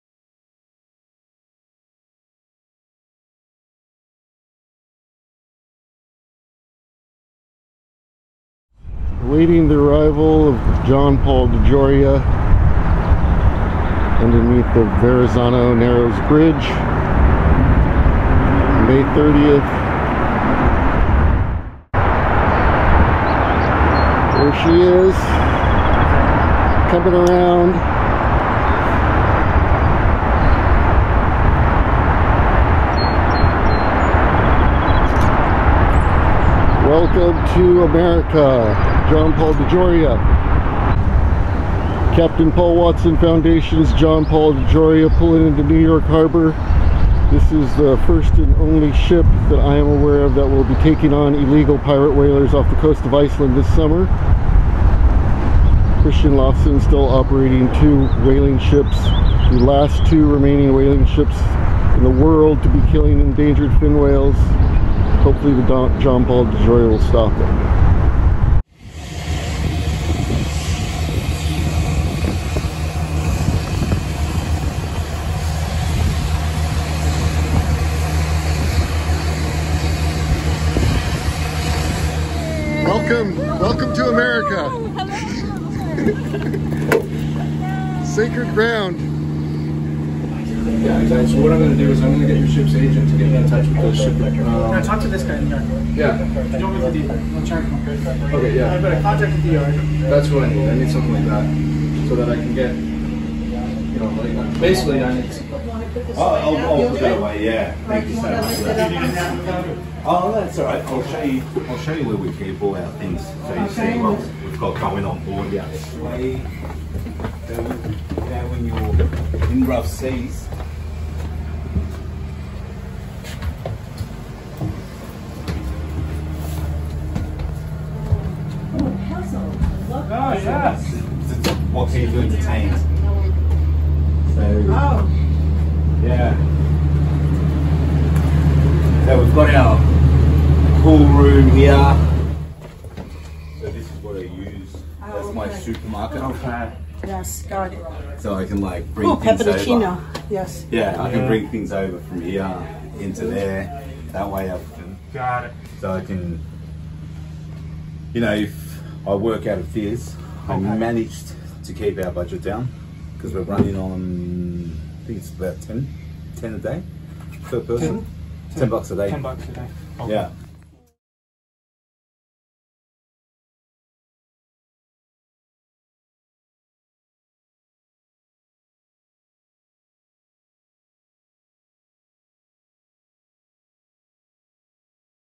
A Visit to the John Paul Dejoria
I only spent a short time with some of the crew yesterday, but I already have gained tremendous respect for their willingness to take a stand for the earth's remaining fin whales. Please help them on this important mission to end a generation of commercial exploitation of the North Atlantic's fin whales by one man, Kristjan Loftsson and his two remaining whaling ships. The only place those two ships belong is on the beach with the other two whaling ships sunk in 1986. No man or his business is as important to this earth as an entire species.

The ship really needs a AED (Automated External Defibrillator) as well as other more common items. Especially if you are in New York! Get in touch and find out how you too can support these amazing humans.